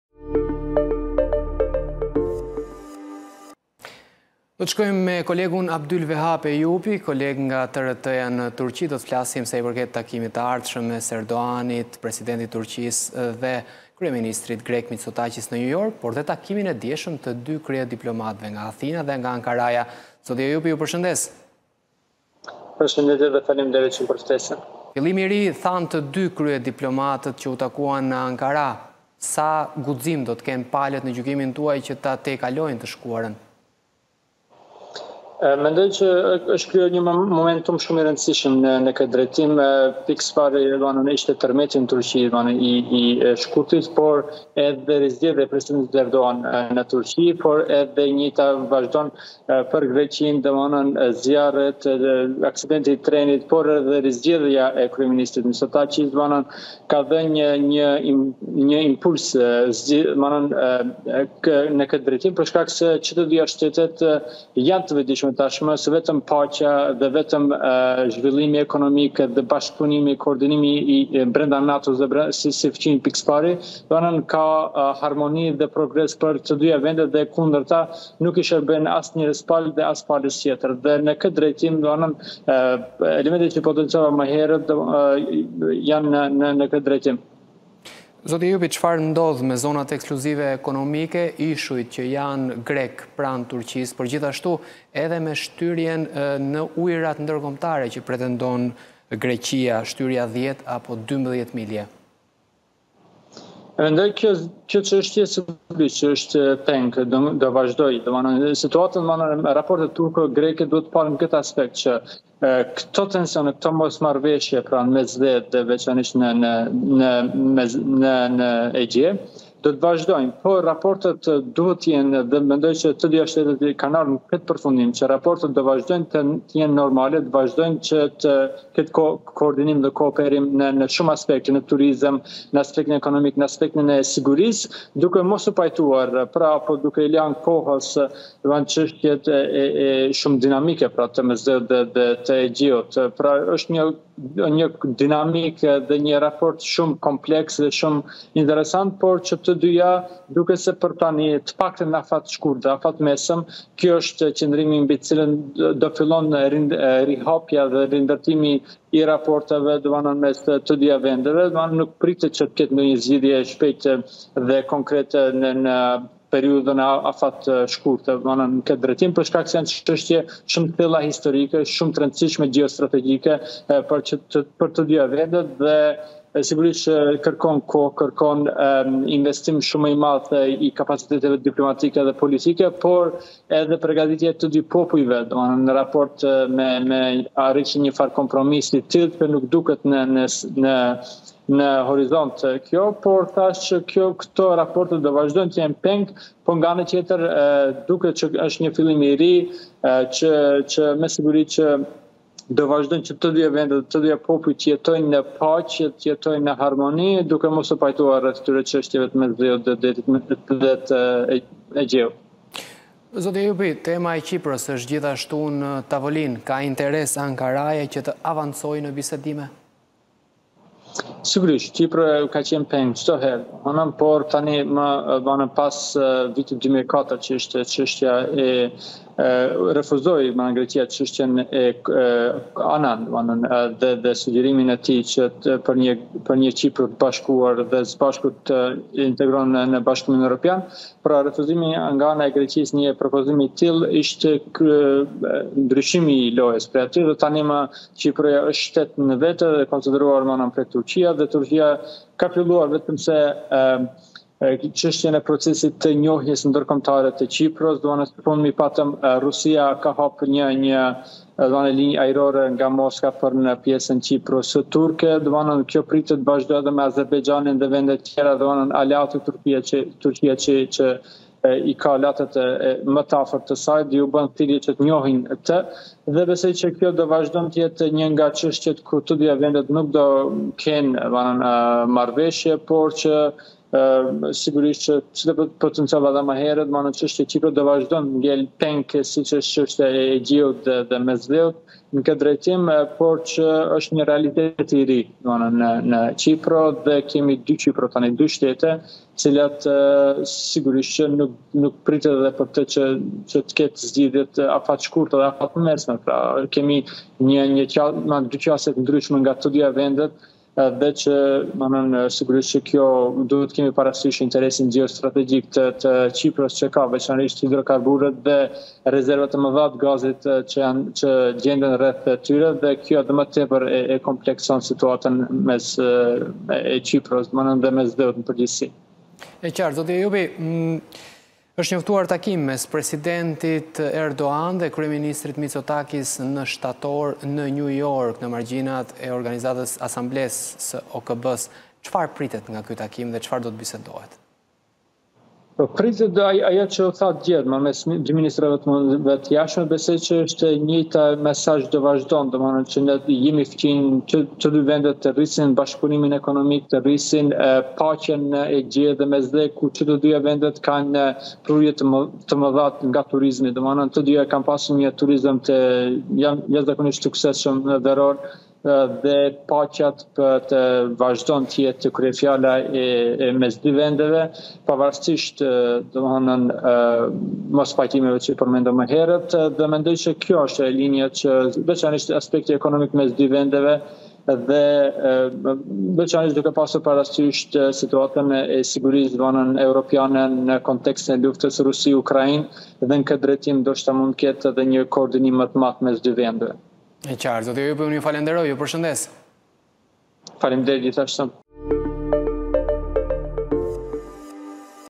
Turqi-Greqi/ Ejupi: Raporte normale në funksion të paqes, mospajtim për çështjet e Egjeut e Mesdheut
Redaktori i TRT Turqi, Abdylvehap Ejupi, foli në edicionin e lajmeve në MCN TV mbi marrëdhënien e re mes Athinës dhe Ankarasë, ku tanimë akujt janë shkrirë.
Ejupi thekson se dy vendet kanë kuptuar rëndësinë e koordinimit ekonomik, në çështje të sigurisë dhe në turizëm. Megjithatë ai vë në dukje se ka mospajtim për çështjet komplekse si Mesdheu dhe Egjeu, apo Qiproja.